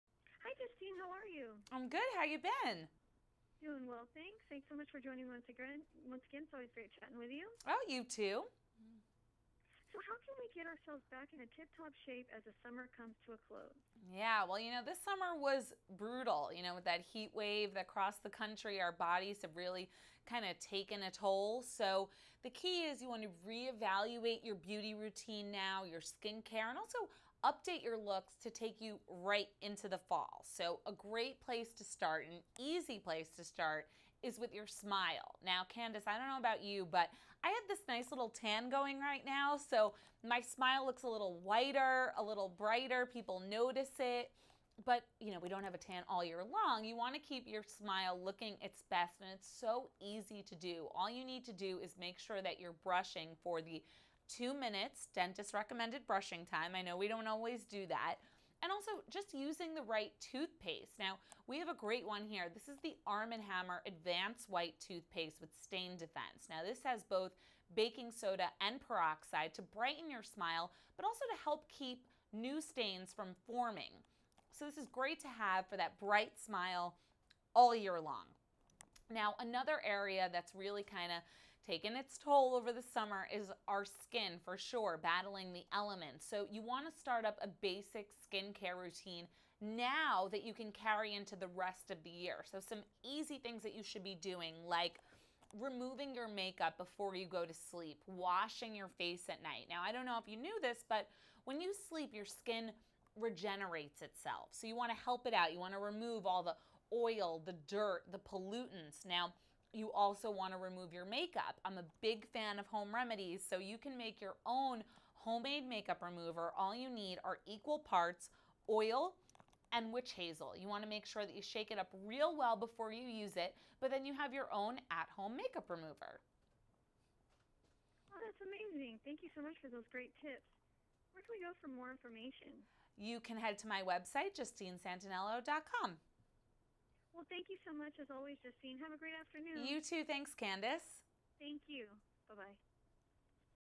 Beauty expert Justine Santaniello dishes the must have beauty products and DIY ideas
Beauty expert Justine Santaniello joined Candace Rose of CandieAnderson.com to dish on the must have beauty products and a homemade makeup remover recipe!